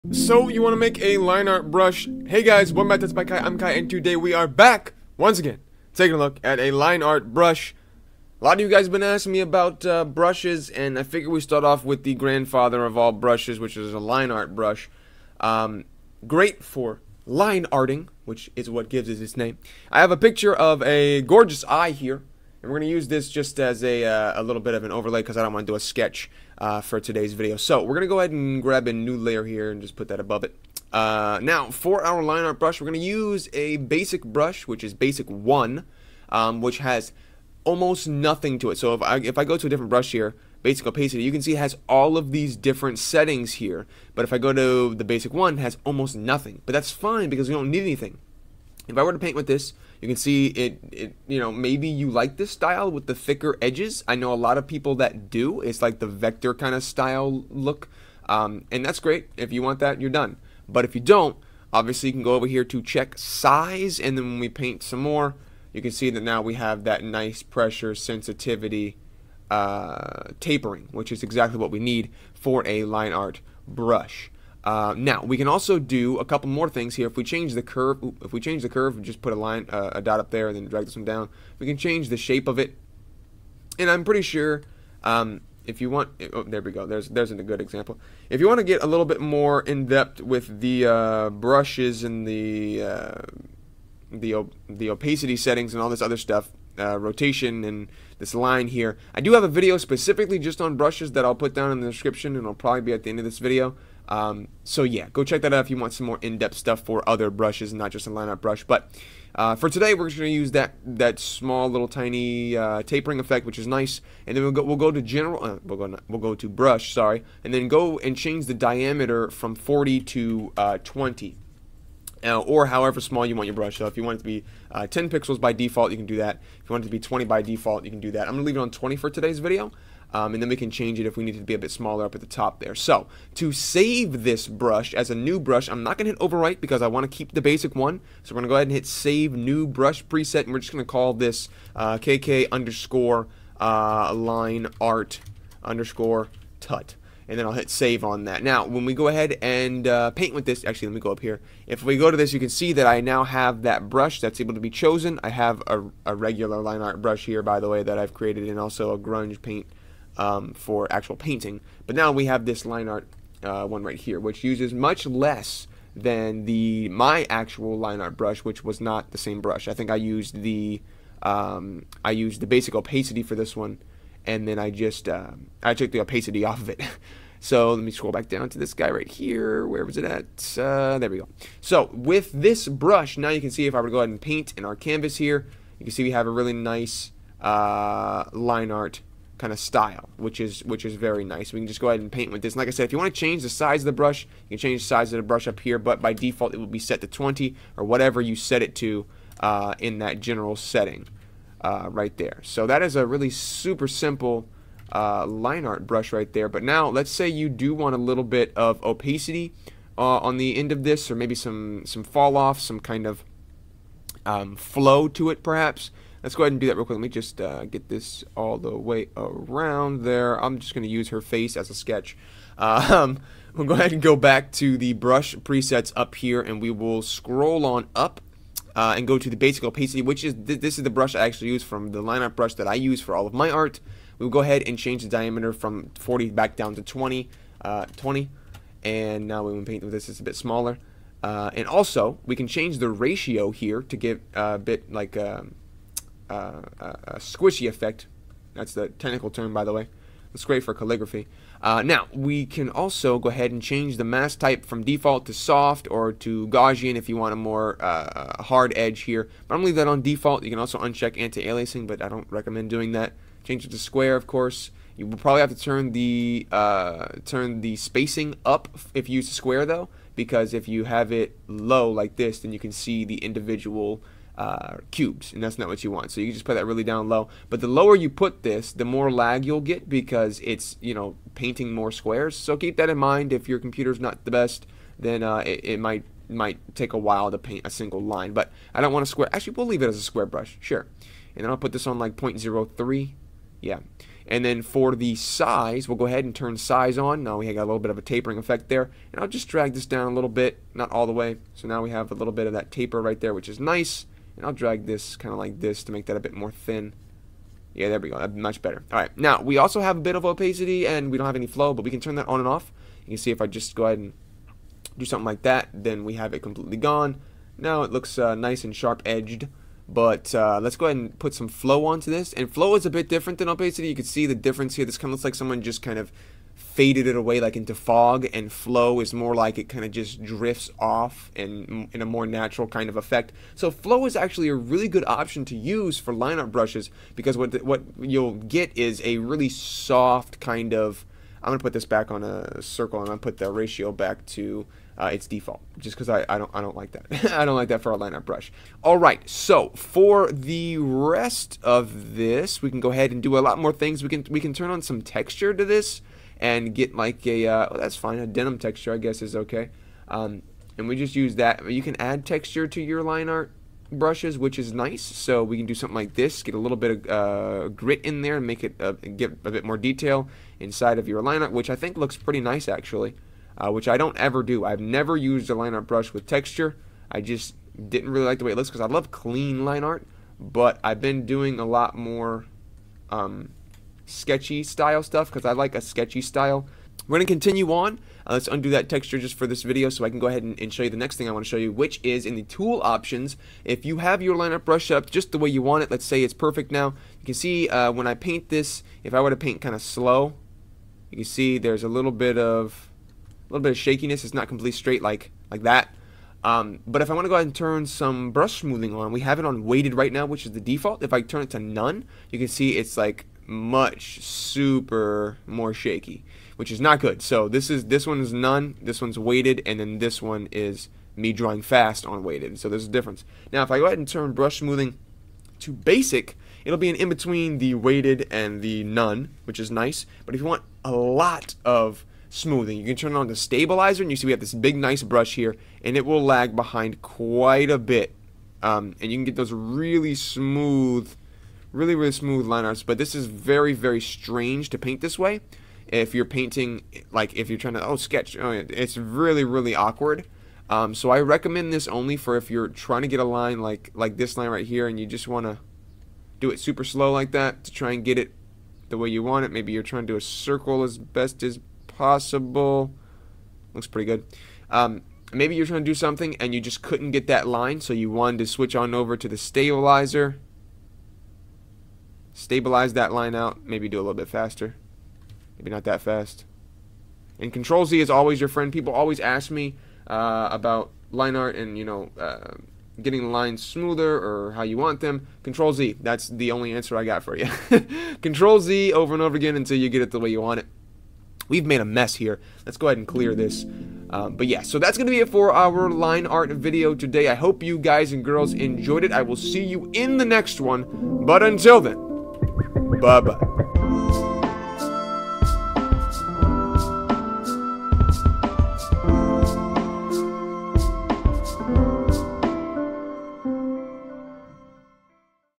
[TUTORIAL] How to create your own LINEART brush in Krita | TutsByKai
Today, we create our own lineart brush in Krita , talk about opacity and flow , pressure sensitivity , brush stabilization and other things that will boost your line art brush game !  :D

▶️ DOWNLOAD MY FREE "asTro" GFX PACK: (coming soon)